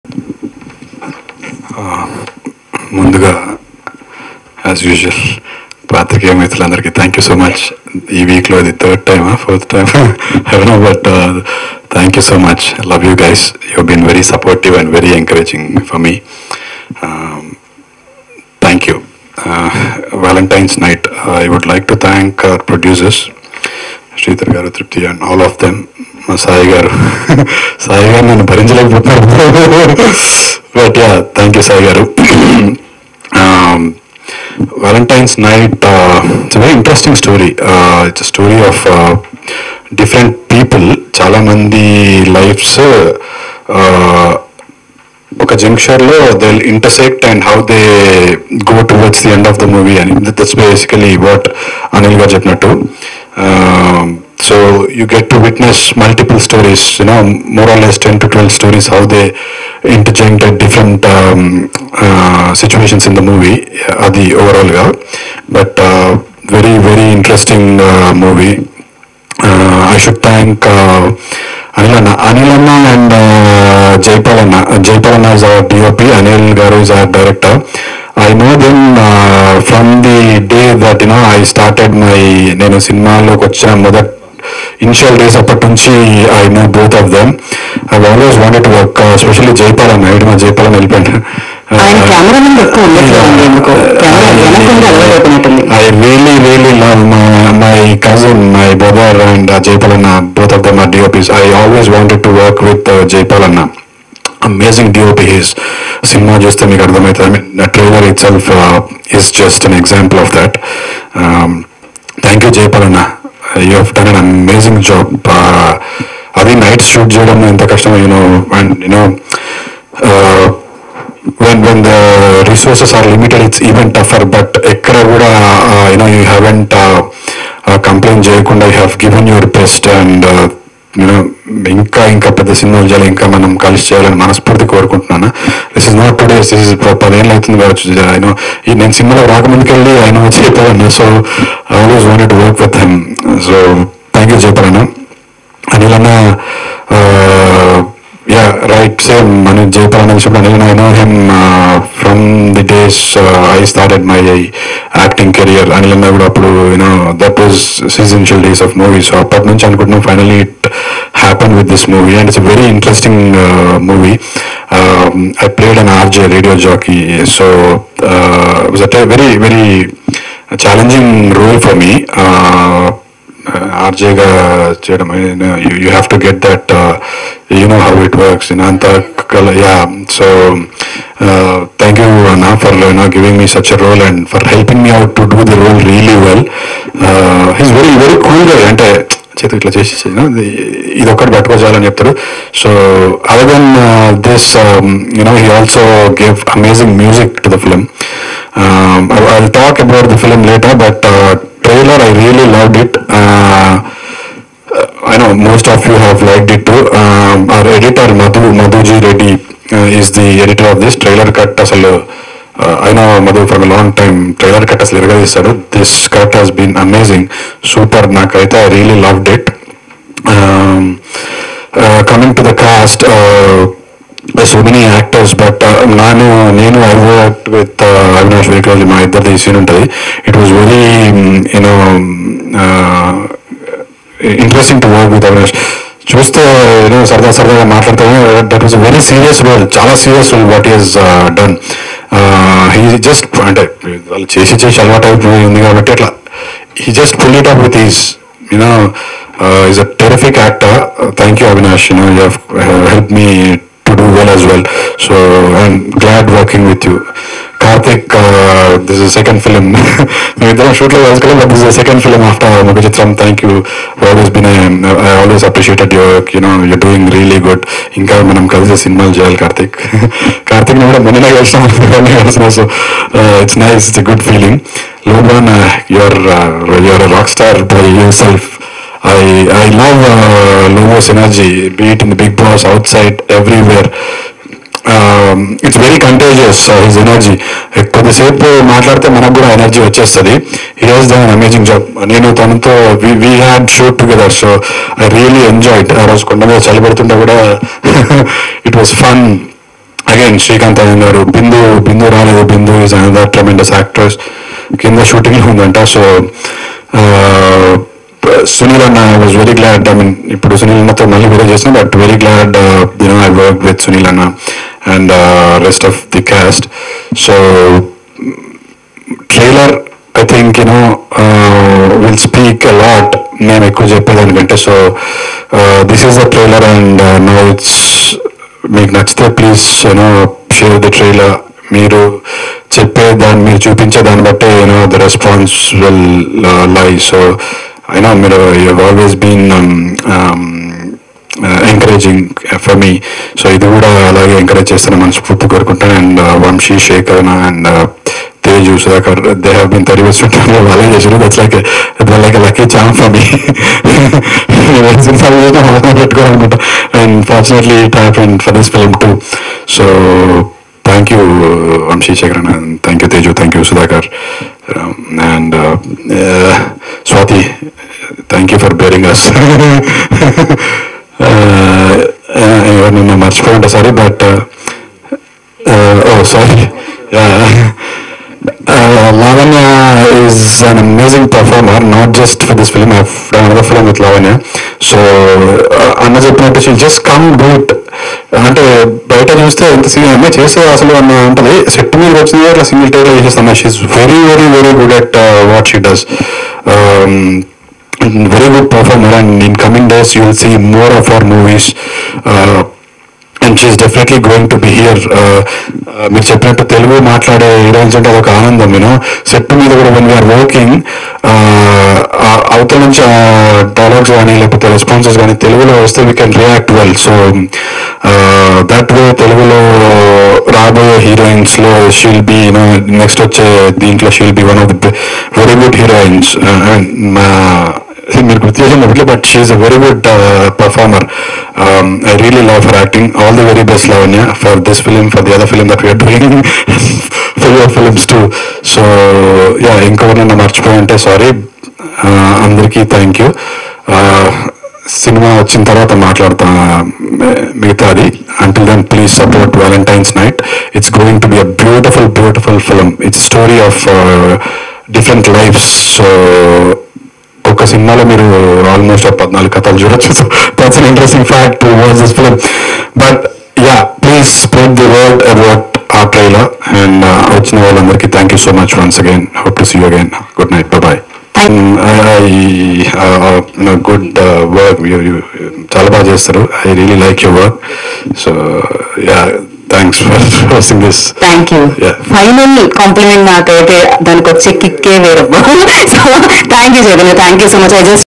30 weds 21 fame chaithanya about his movie | valantines night trailer launch | friday poster
30 weds 21 fame chaithanya about his movie | valantines night trailer launch | friday poster